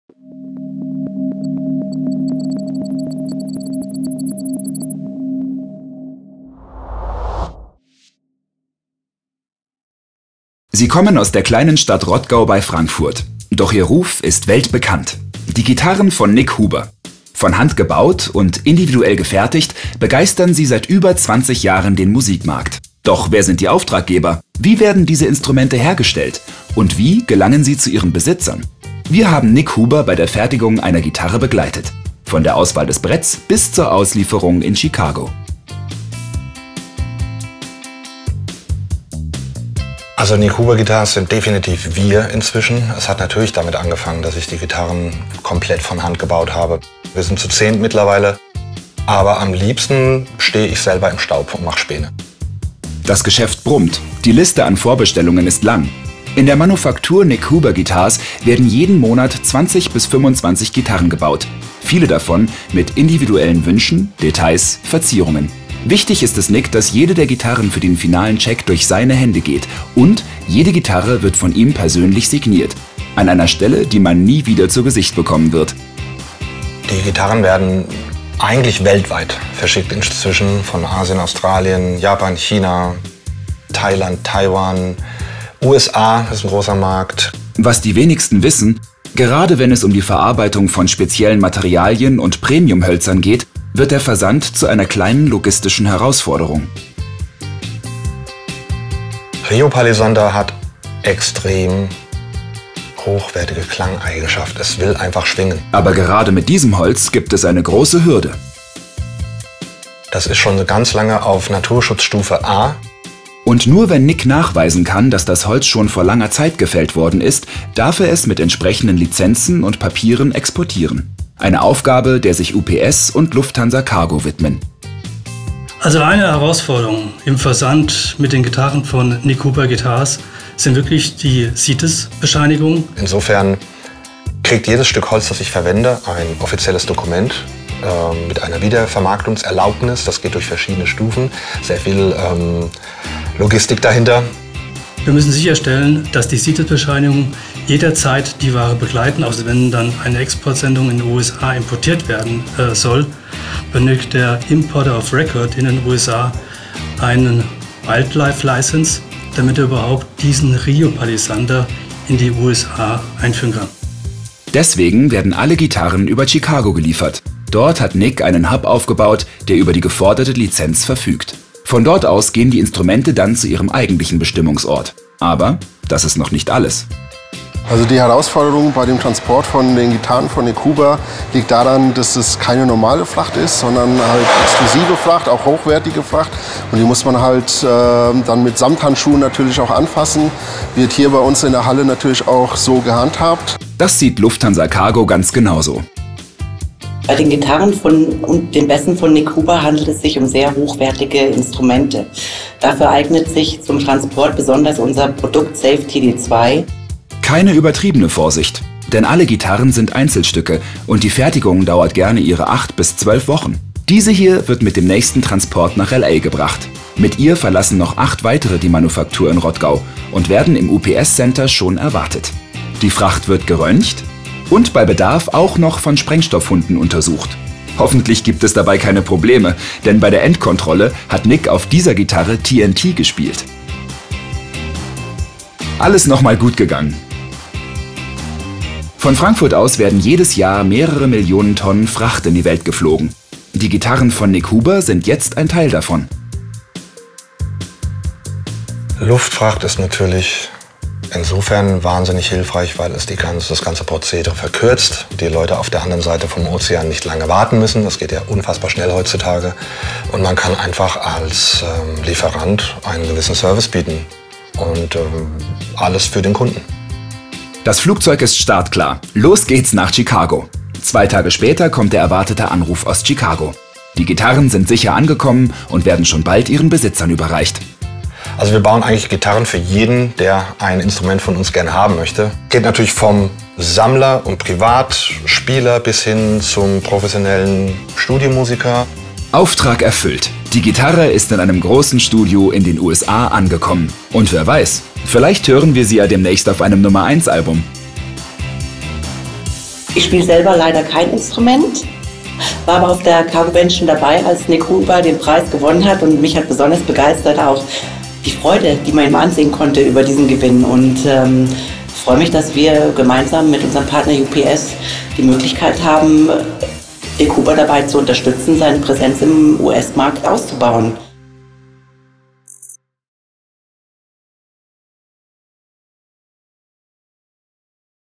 HessenGoesGlobal Finalist 2018 - Nik Huber Guitars: Update
Music for the win: Here comes our second 'HessenGoesGlobal' winner, Nik Huber guitars. Find out how UPS helps us deal with the challenges of shipping these instruments handmade from rare woods to the US and all over the world in the video.